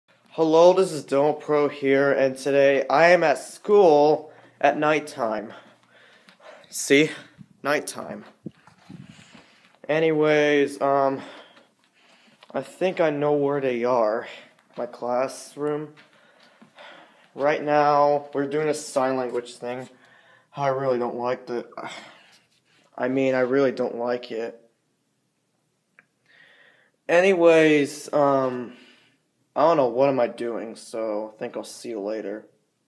School in night time
I was at school at night because i was doing a sign language concert last night